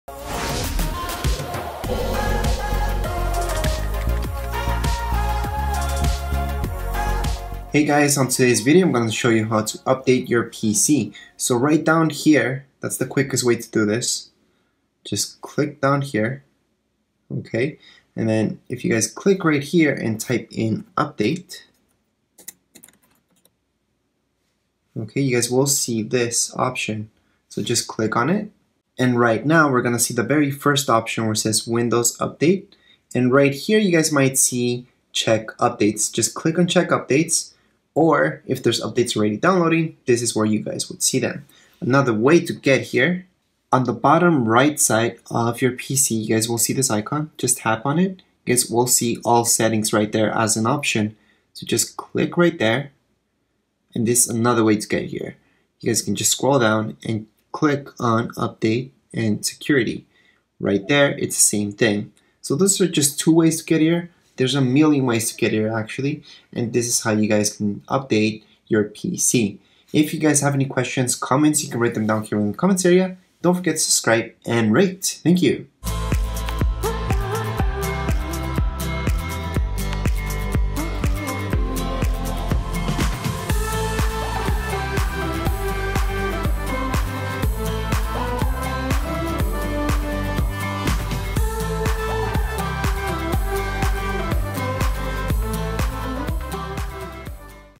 How to Keep your Windows Computer up-to-date | Windows 10 | how to run System Updates on Pc
How to Update windows 10